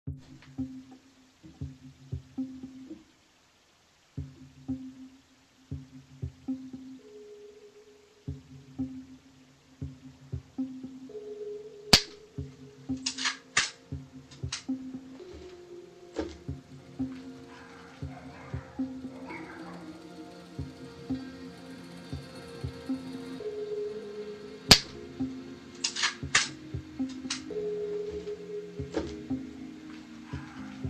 CAUGHT ON CAMERA: Camouflaged man points gun at North Carolina home
Chilling security footage captures the moment a man dressed in camouflage stood on the porch of a McDowell County, North Carolina, home and pointed a gun toward a window.